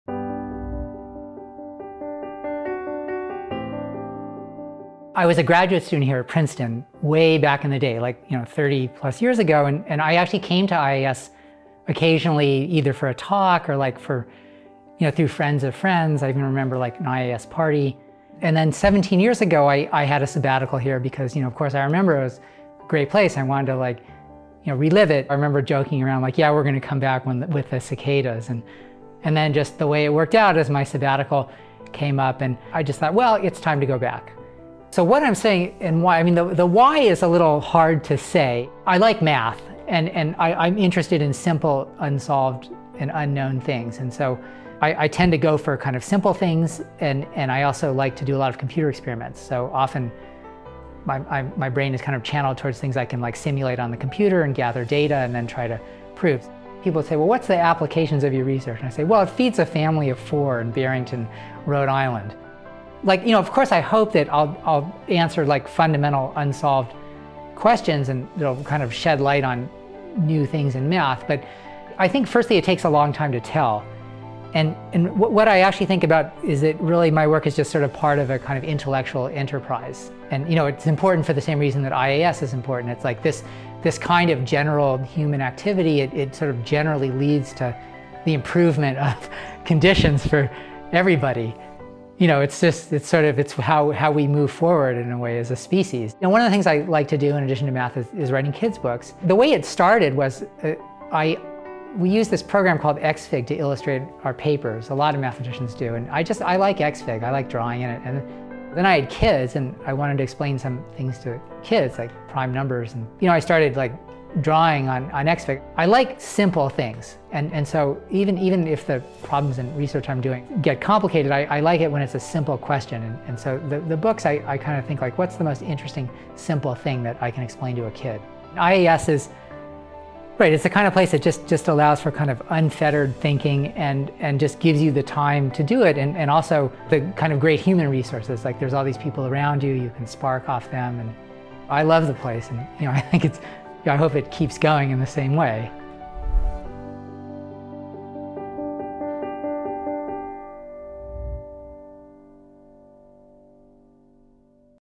Scholar Spotlight: Richard Schwartz
In this Scholar Spotlight we feature Richard Schwartz who was a Member in the School of Mathematics earlier this year. He talks about his interest in fundamental questions in math and how the Institute’s commitment to curiosity-driven research can make our society better. Beyond solving complex problems, Rich has a passion for writing children’s books to explain mathematical concepts.

Support the IAS mission of advancing academic freedom and curiosity-driven research by visiting

Scholar Spotlight is produced by OFC (ofc.tv)